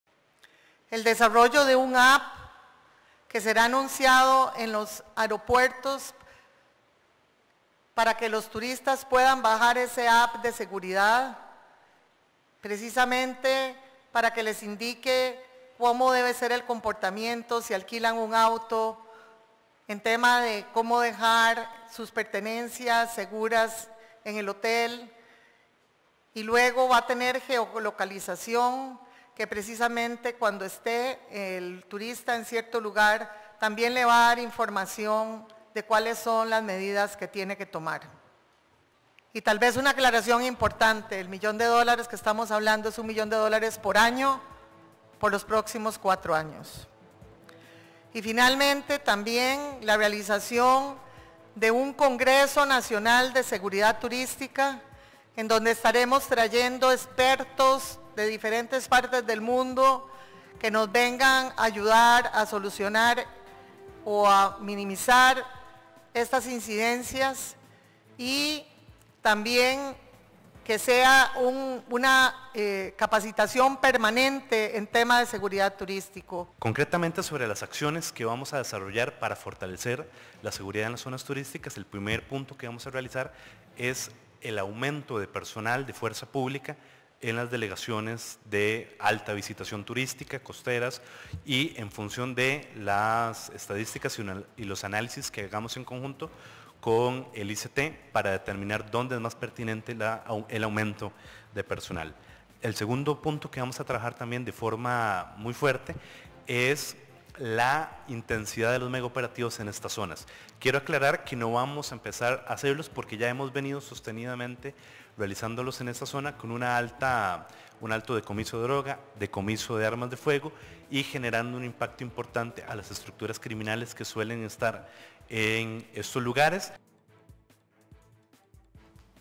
Instituto Costarricense de Turismo lanzará aplicación móvil para informar y cuidar visitantes
El Instituto Costarricense de Turismo; ICT, lanzará una aplicación para ciudadanos de este país centroamericano y extranjeros que visiten esta nación cuya primera etapa brindará consejos en materia de seguridad.

El ICT implementará una ruta de trabajo para que esta aplicación pueda estar funcionando en diciembre, al comienzo de la temporada alta en suelo costarricense.

La aplicación será de descarga gratuita tanto para dispositivos con el sistema operativo Android como con iOS y orientará al turista en materia de seguridad, así como en itinerarios, sugerencias de los más llamativos espectáculos de la naturaleza, entre otros.

Además, incluirá información de embajadas, enlaces a otras apps útiles como por ejemplo de sismos y a sitios web con servicios como el Instituto Meteorológico.

La institución turística además capacitará a 2 mil empresarios del sector en los próximos seis meses en estrategias de prevención.

Las autoridades costarricenses que implementan estas acciones tras los recientes hechos de violencia reciente contra visitantes.

Esta son unas  de las medidas integrales dadas a conocer este viernes en Casa Presidencial de Costa Rica por la ministra de Turismo, María Amalia Revelo, y el viceministro de Seguridad Pública, Eduardo Solano.

En la actividad, los jerarcas informaron que la Comisión Nacional de Seguridad Turística; CONSETUR, perfilará el plan de inversión de $1 millón anuales del próximo cuatrienio que el ICT destinará a la seguridad turística mediante una alianza con el Ministerio de Seguridad Pública.

La CONSETUR la conforman el Ministerio de Seguridad Pública, Organismo de Investigación Judicial, Fiscalía General de la República, ICT, y representantes de la Embajada de Estados Unidos, la Cámara Nacional de Turismo; Canatur y la Cámara de Turismo de Guanacaste; Caturgua.

Además, el viceministro de Seguridad, Eduardo Solano informó que se va a reforzar con personal de la Policía Turística y que, paralelamente, se harán mega operativos en algunos de los sitios de mayor visitación turística.

El gobierno costarricense también realizará una campaña de sensibilización con perspectiva de género, que incluirá entre sus objetivos un recordatorio a la ciudadanía de que la seguridad la construimos en conjunto, recalcará el respeto y dignidad de las mujeres, llamará a cuidarnos unos a otros y también a los turistas que nos visitan.

Además este país centroamericano realizará el Congreso de Seguridad Turística, que reunirá a expertos en el tema, costarricenses y extranjeros, para que compartan casos de éxito, ejemplarizantes e inspiradores para el sector turístico de esta nación.